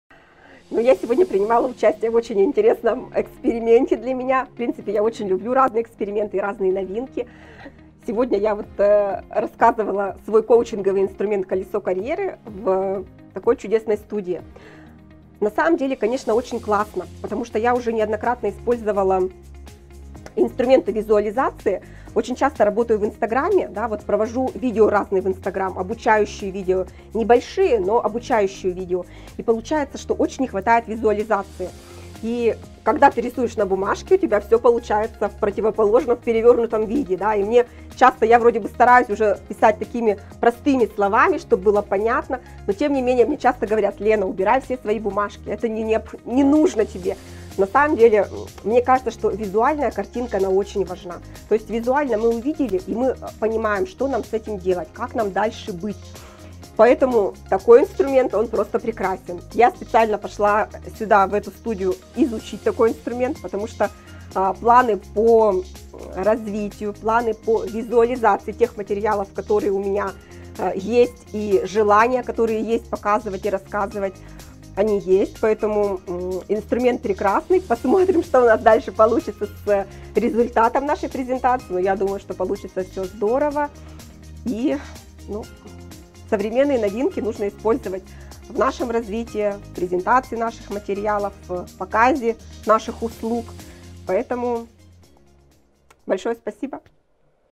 Отзыв о первой записи в студии видеозаписи с прозрачной видео доской
Ура! Новый отзыв о записи в нашей студии!

Приходите к нам в студию! Первый час записи - БЕСПЛАТНО!

Как связаться?